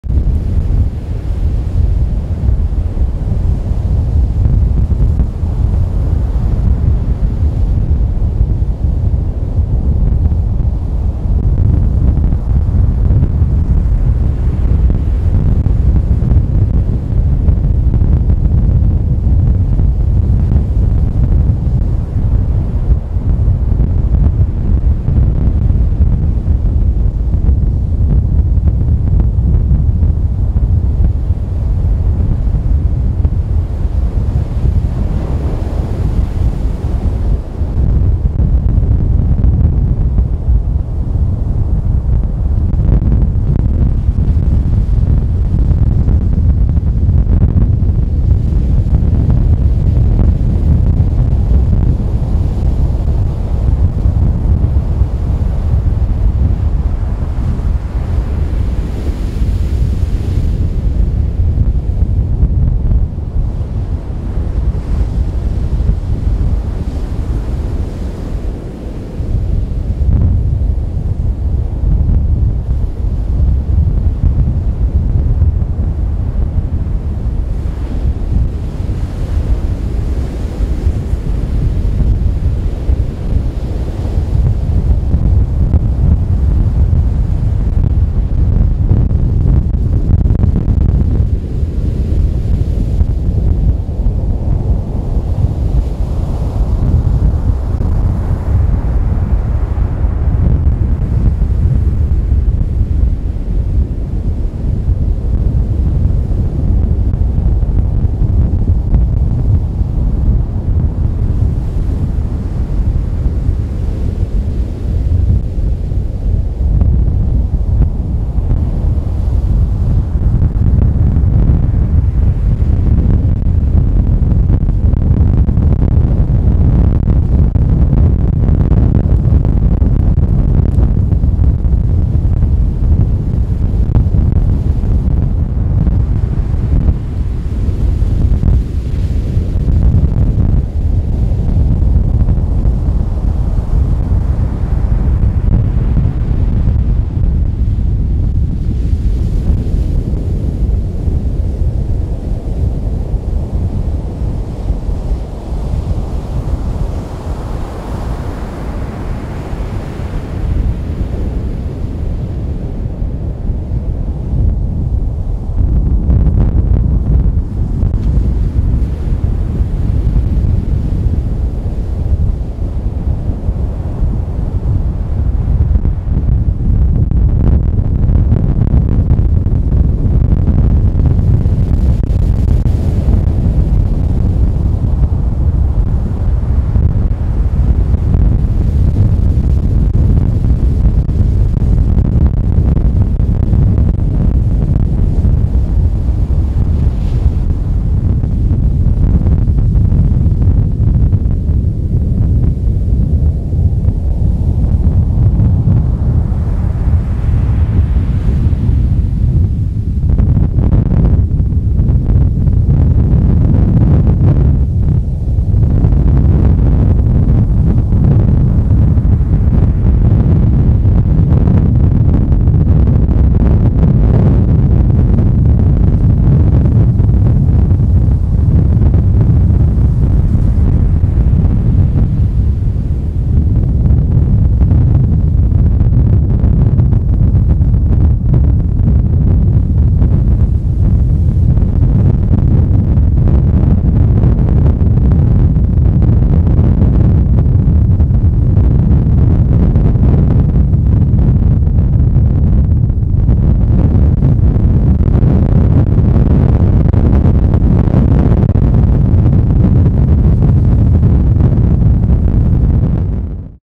1/15/2022
Good morning!  1/15/2022

23° on the beach with a strong N wind 13-23mph

Ocean Water temp at the Inlet Buoy reads 41°